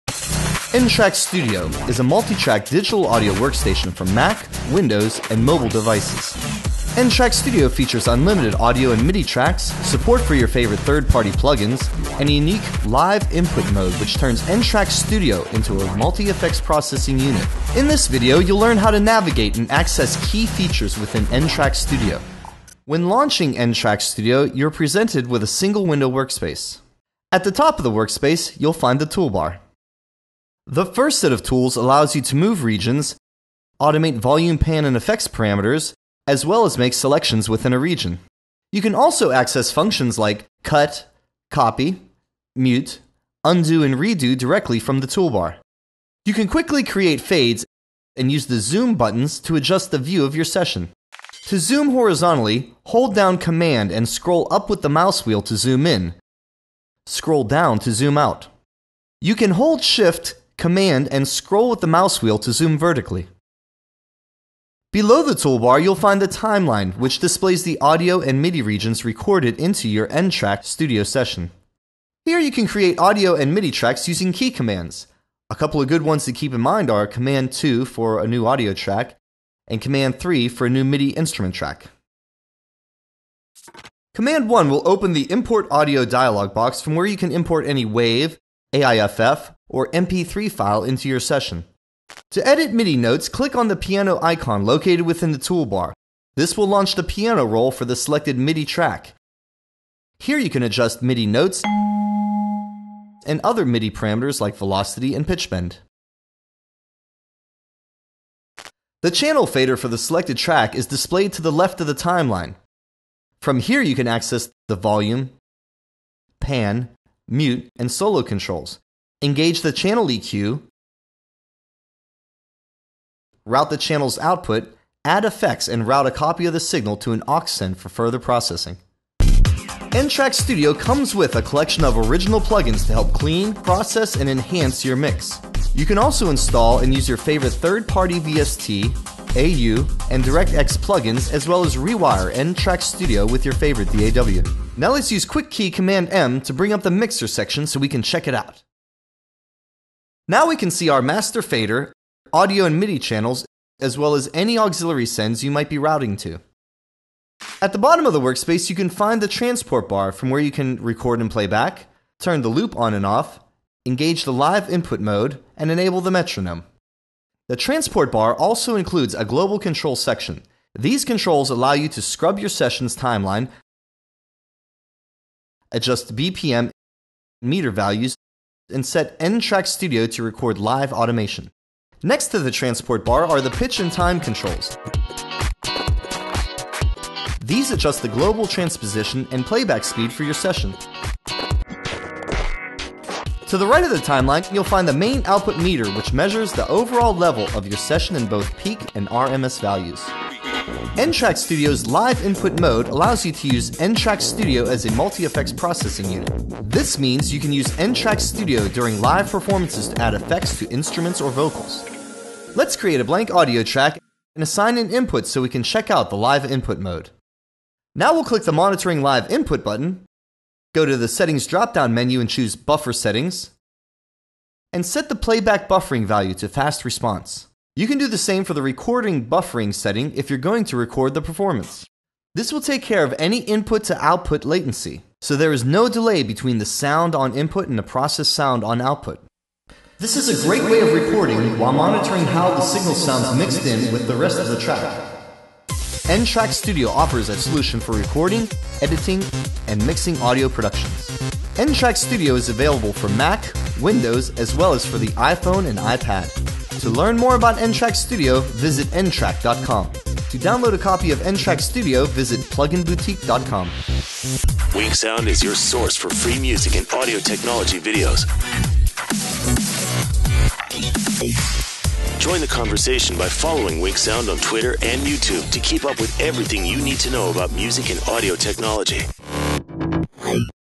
n-Track Studio: Overview of n-Track Studio 7 | WinkSound
n-Track Studio 7 is a multitrack music production software featuring unlimited audio and MIDI tracks, support for your favorite third party plugins and a unique Live Input Mode which turns n-Track Studio into a multi-effects processing unit. In this video learn how to navigate and access key functions within n-Track Studio 7.

To use n-Track Studio's Live Input Mode do the following:

1. Create a new audio track.

2. Assign an input.

3. Turn on the "Monitoring Live Input" button on the audio track.

4. To ensure there is no latency between the signal on input and the affected signal on output     go to the Settings drop down at the top of the workspace and choose "Buffer Settings".

5. Set the Playback Buffer to "Fast Response". You can do the same with the Record Buffer if you     are going to record the performance.

6. Now when you send an signal to the input n-Track Studio 7 will process the audio and send it     directly to the output bus for instant monitoring.